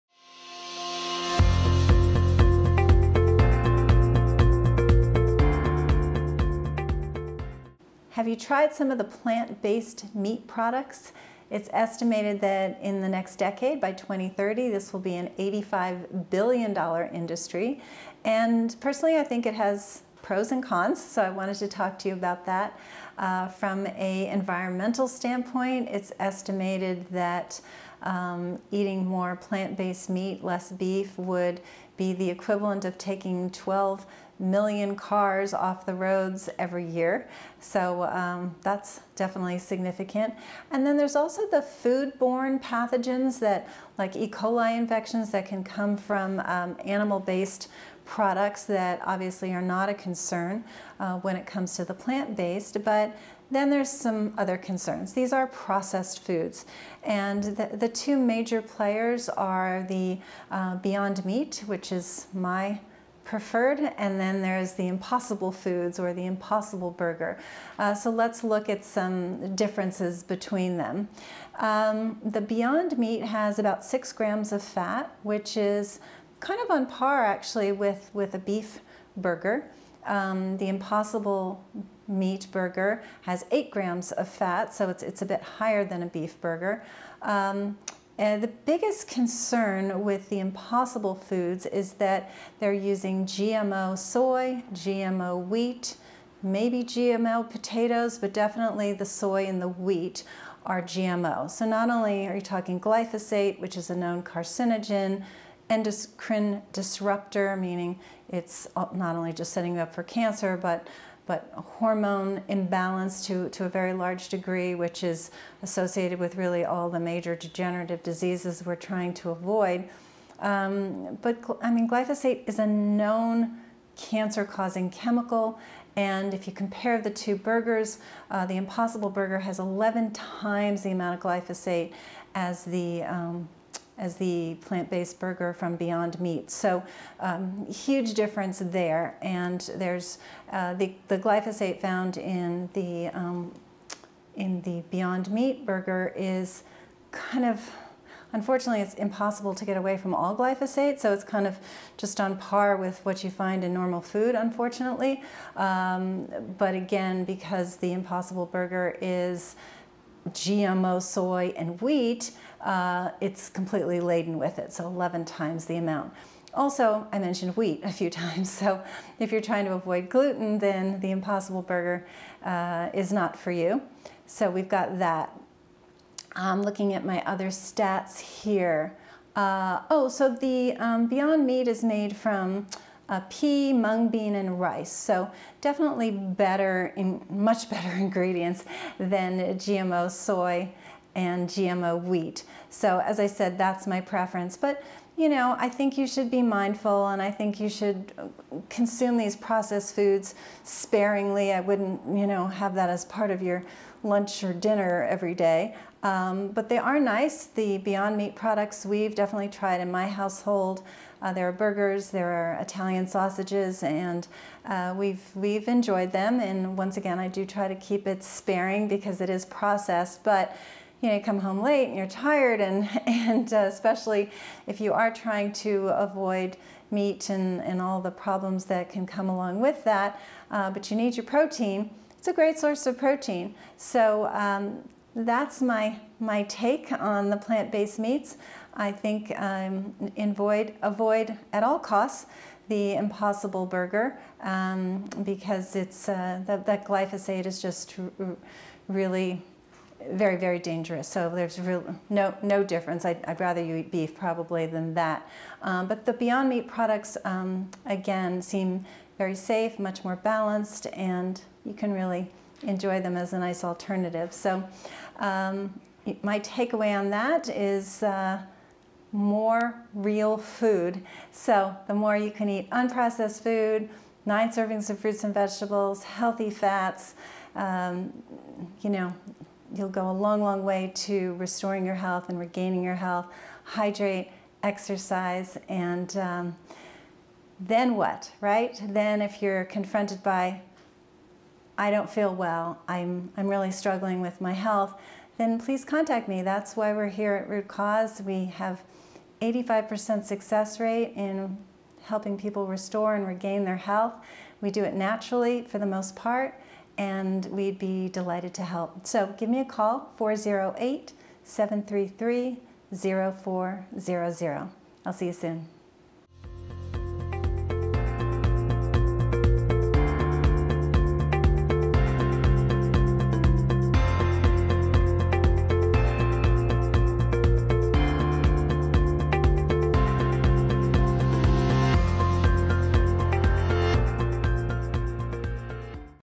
Are Plant Based Meat Alternatives Healthy?
The plant based meat industry is growing like crazy, estimated to reach $85 billion within the decade.
The major "players" currently are Beyond Meat and Impossible Foods.
The nutrition content is very different between the two, and while I think processed foods should overall be kept to a minimum, I have a definite preference if you choose to use alternative meats as a quick meal option.
Give a listen to learn more.